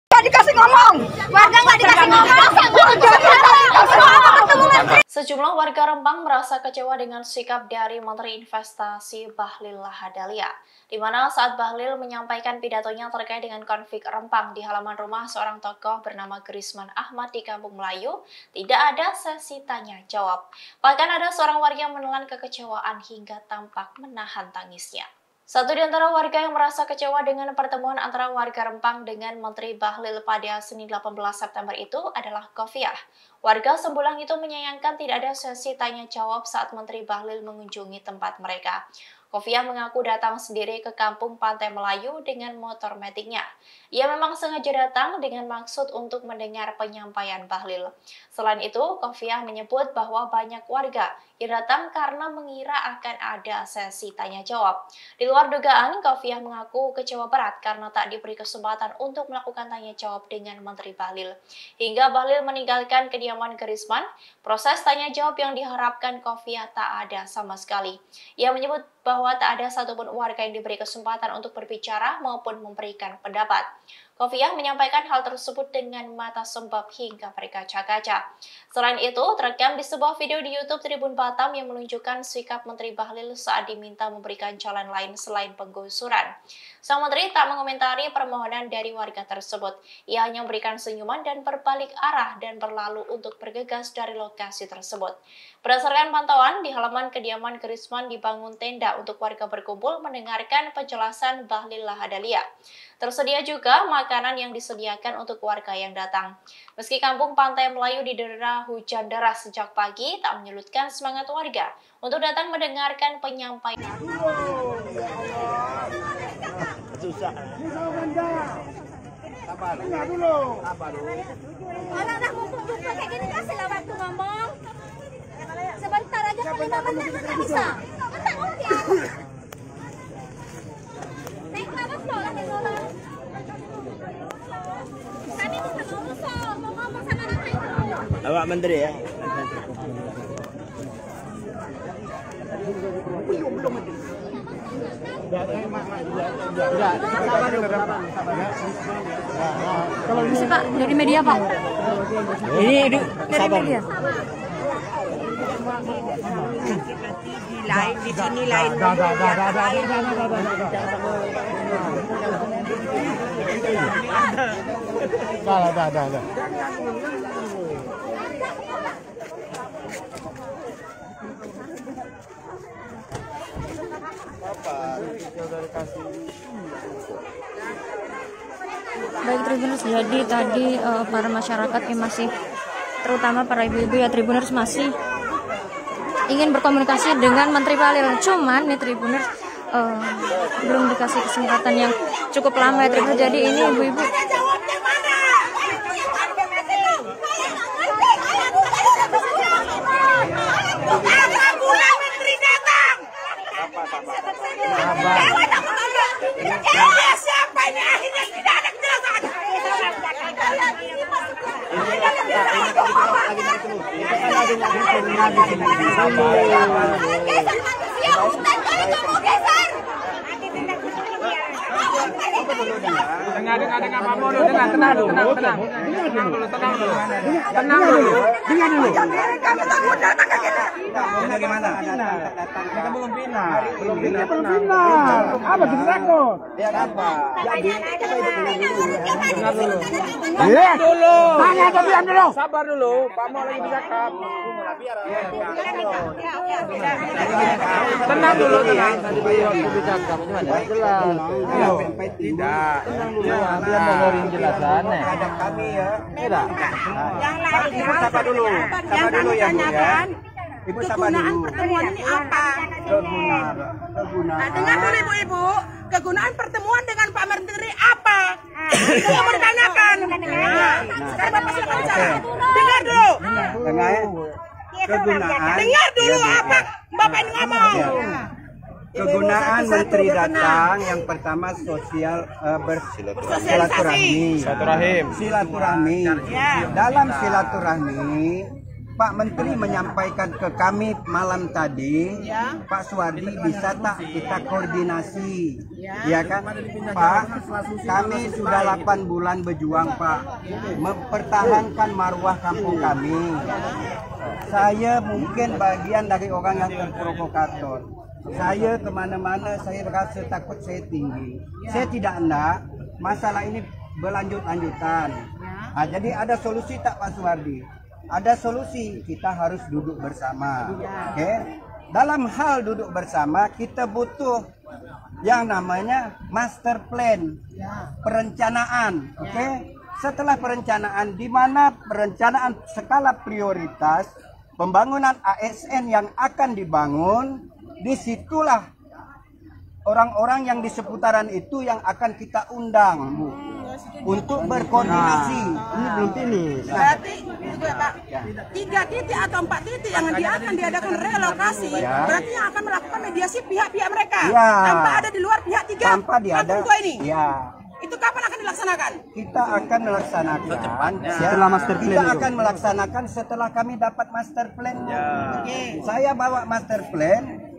PENGAKUAN Warga Rempang 'Diacuhkan' Menteri Bahlil: Kami Pikir Ada Tanya Jawab Makanya Warga Datang
TRIBUN-VIDEO.COM - Sejumlah warga Rempang merasa kecewa dengan sikap dari Menteri Investasi Bahlil Lahadalia.

Di mana saat Bahlil menyampaikan pidatonya terkait dengan konflik di Rempang di halaman rumah seorang tokoh Gerisman Ahmad di Kampung Melayu, tidak ada sesi tanya jawab.

Bahkan ada seorang warga yang menelan kekecewaan hingga tampak menahan tangisnya.

Satu di antara warga yang merasa kecewa dengan pertemuan antara warga Rempang dengan Menteri Bahlil pada Senin (18/9/2023) itu adalah Khofiah.

Warga Sembulang itu menyayangkan tidak ada sesi tanya jawab saat Menteri bahlil mengunjungi tempat mereka.

Khofiah mengaku datang sendiri ke Kampung Pantai Melayu dengan motor matic.

Ia memang sengaja datang dengan maksud untuk mendengar penyampaikan Bahlil.

Selain itu, Khofiah menyebut bahwa banyak warga yang datang karena mengira akan ada sesi tanya jawab.

"Saya sengaja datang ke sini dengan maksud mendengar penyampaian pak Bahlil, Menteri Investasi, kami pikir ada tanya jawab dengan warga makanya datang,"kata Khofiah kepada TribunBatam.id.

Di luar dugaan, Khofiah mengaku kecewa berat karena tak diberi kesempatan untuk melakukan tanya jawab dengan Menteri Bahlil.

Hingga Bahlil meninggalkan kediaman Gerisman, proses tanya jawab yang diharapkan Khofiah tak ada sama sekali.

Ia menyebut bahwa tak ada satupun warga yang diberi kesempatan untuk berbicara maupun memberikan pendapat.

Khofiah menyampaikan hal tersebut dengan mata sembab hingga berkaca-kaca.

"Tapi rupanya tidak diberi kesempatan warga ngomong, kasih pendapat," katanya dengan mata sembab berkaca-kaca.

Selain itu, terekam di sebuah video di YouTube TribunBatam yang menunjukkan sikap Menteri Bahlil saat diminta memberikan jalan lain selain penggusuran.

Sang menteri tak mengomentari permohonan dari warga tersebut.

Ia hanya memberikan senyuman dan berbalik arah dan berlalu untuk bergegas dari lokasi tersebut.

Berdasarkan pantauan, di halaman kediaman Gerisman dibangun tenda untuk warga berkumpul mendengarkan penjelasan Bahlil Lahadia.

Tersedia juga makanan yang disediakan untuk warga yang datang.

Meski kampung Pantai Melayu didera hujan deras sejak pagi, tak menyulutkan semangat warga untuk datang mendengarkan penyampaian dari Menteri Bahlil.

(Tribun-Video.com/TribunBatam.id)

Artikel ini telah tayang di TribunBatam.id dengan judul Khofiah Berkaca-kaca Tak Bisa Dialog dengan Menteri Bahlil Lahadalia di Rempang,
Penulis: Aminudin | Editor: Septyan Mulia Rohman